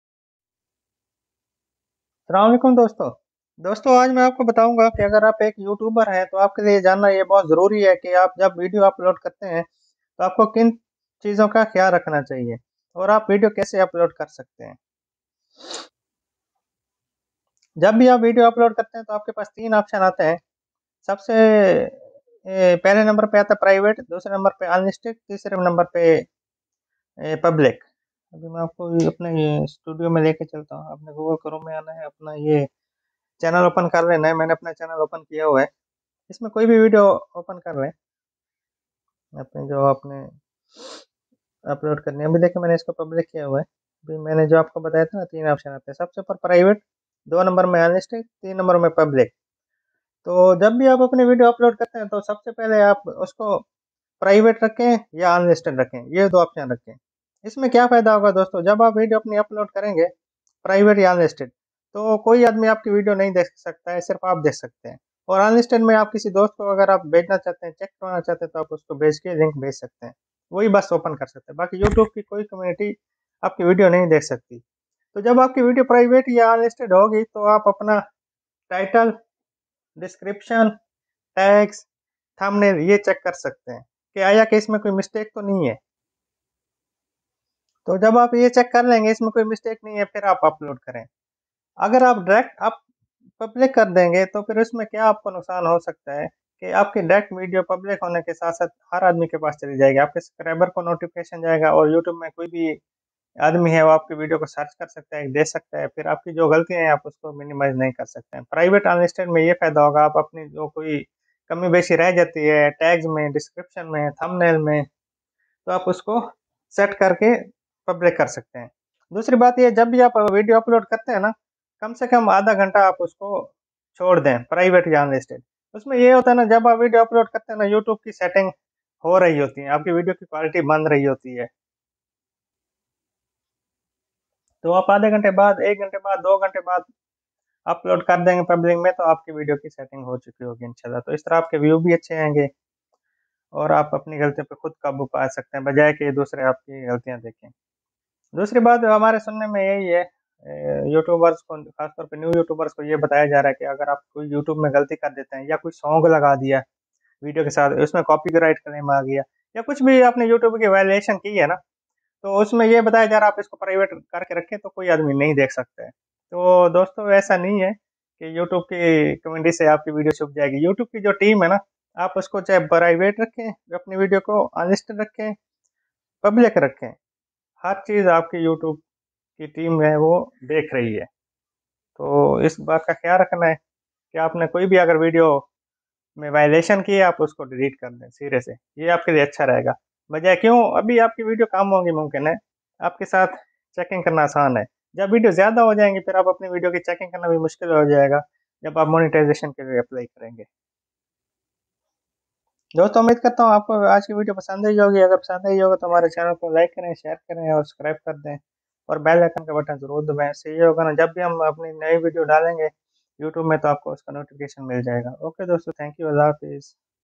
what is the difference between public and unlisted on youtube
Today in this video I will guide you that how you should upload video on youtube